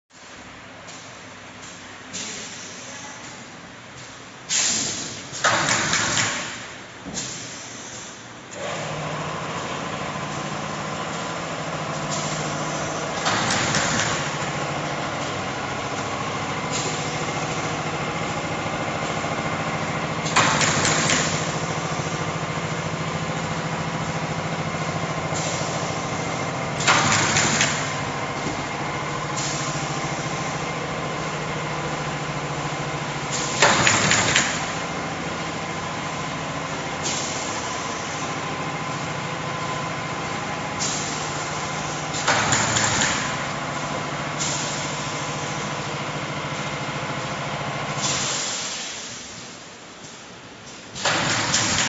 Second hand automatic folder glue machine
This type machine are used to produce small carton box.
If you need or interested in kindly contact with me:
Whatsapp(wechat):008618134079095